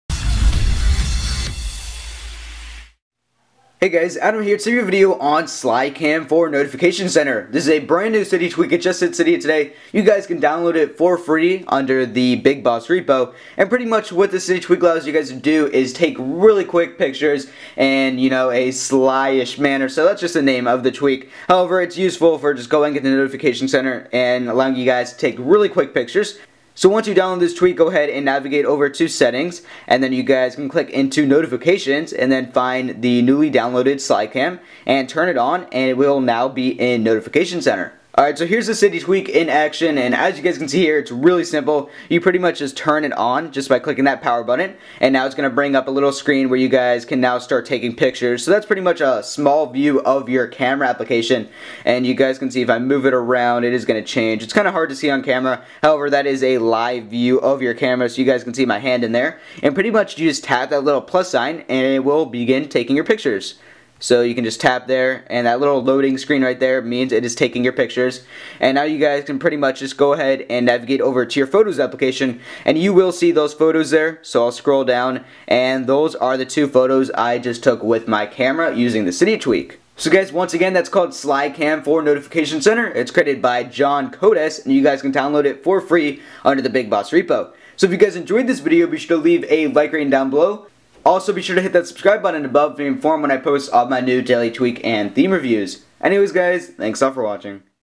Take Photos from Notification Center with 'SlyCam for NotificationCenter'
Take Quick Photos Right from Notification Center with 'SlyCam for NotificationCenter' (Cydia Tweak)

Name: SlyCam for Notification Center
Price: Free
Repo: BigBoss

SlyCam for Notification Center is a Cydia tweak that allows you to take super quick photos, right from notification center. The Cydia tweak is free, just search it up and Cydia and you'll be able to find it under the BigBoss repo. Once you install the tweak, navigate over to settings then notification center and find the tweak. You can then enable it from there and it will then be in your notification center. Inside notification center, the tweak is really simple. Click the power button to enable the tweak and then you can then click the '+' to take a picture. There's the live preview right to the side of this, making it great for super quick photo taking.